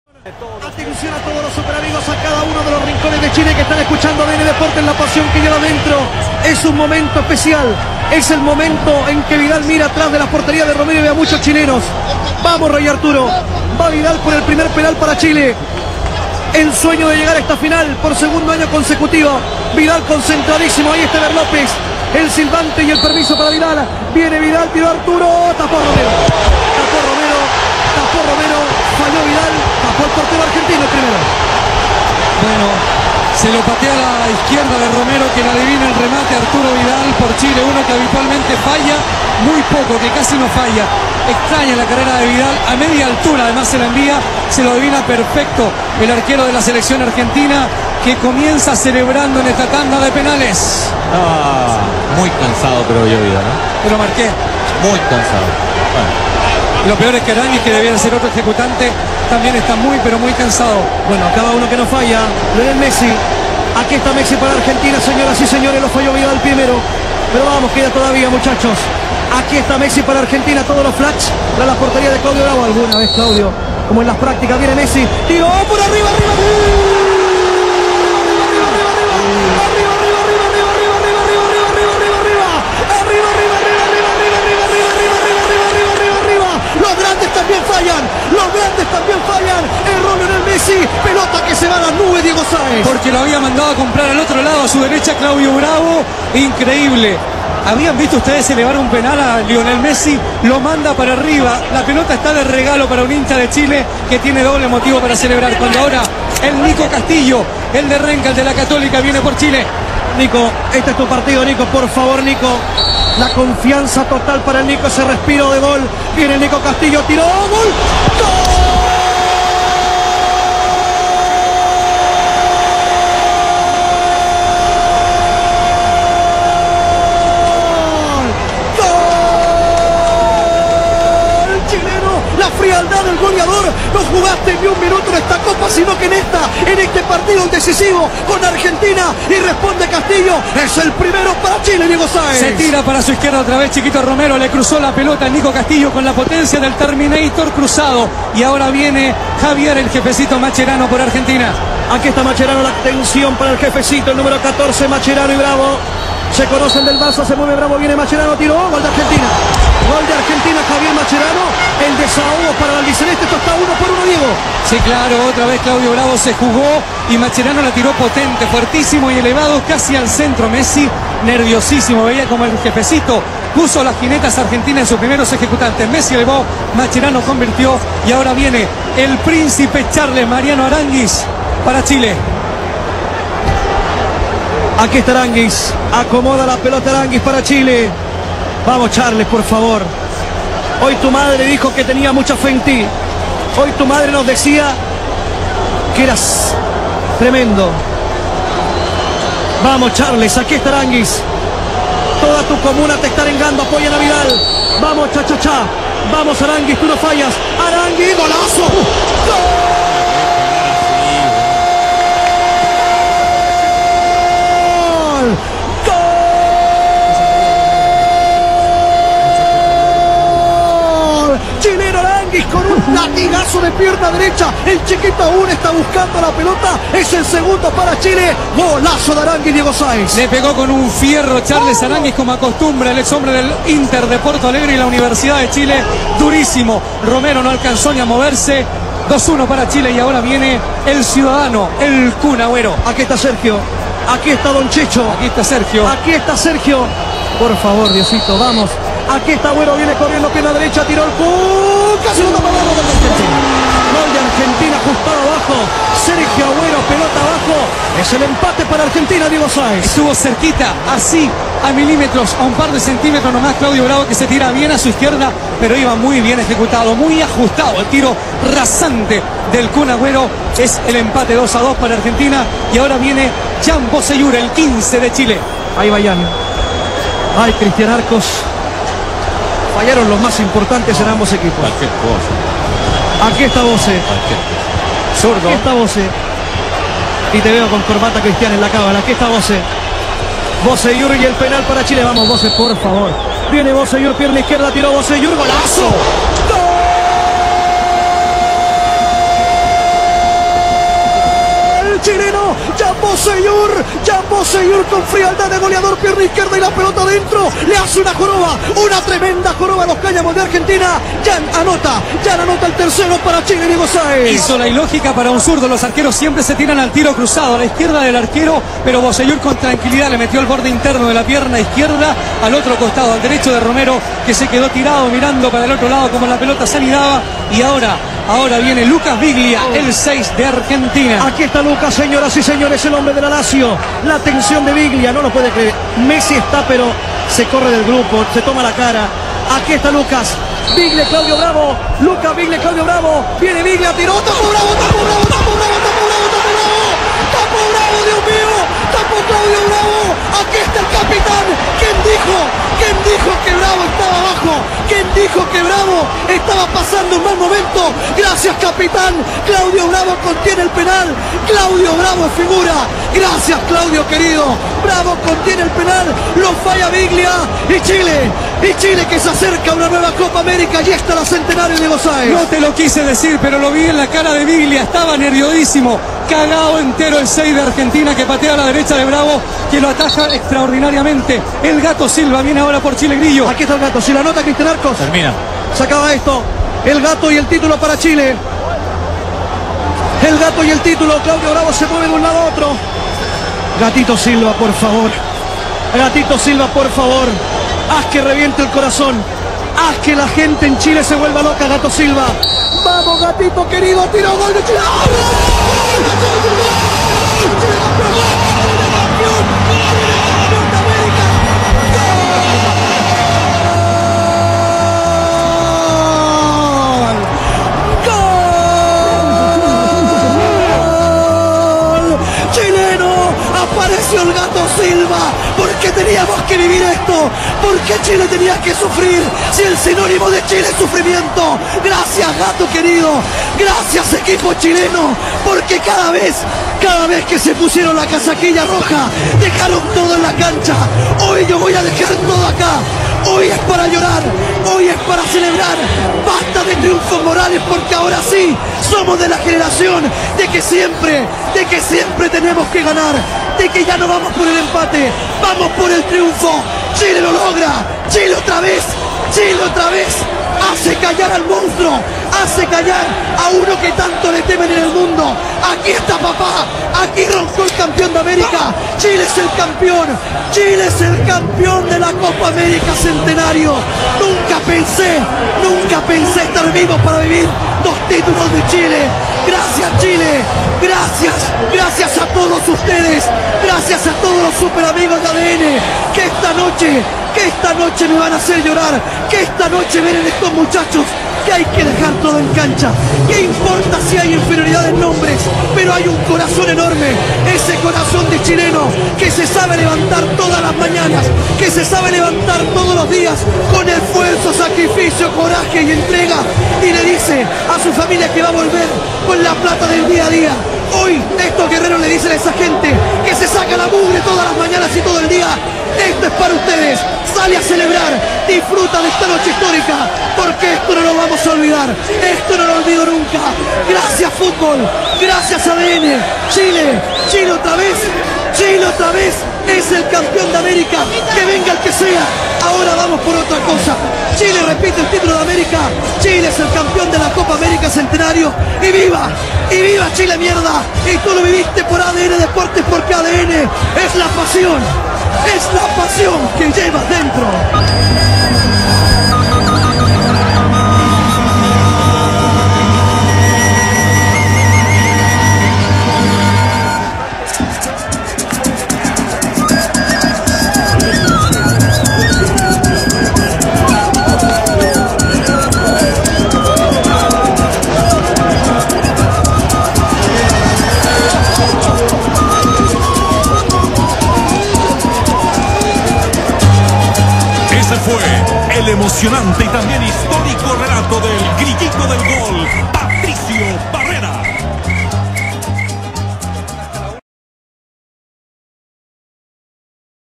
⚽Emocionante Relato Chileno Fullᴴᴰ ►Final Argentina vs Chile Copa América Centenario 2016
Relato emocionante radio ADN Chile. 26 junio de 2016. Chile Campeón Copa América Centenario.

Edición: Davisito de Zabedrosky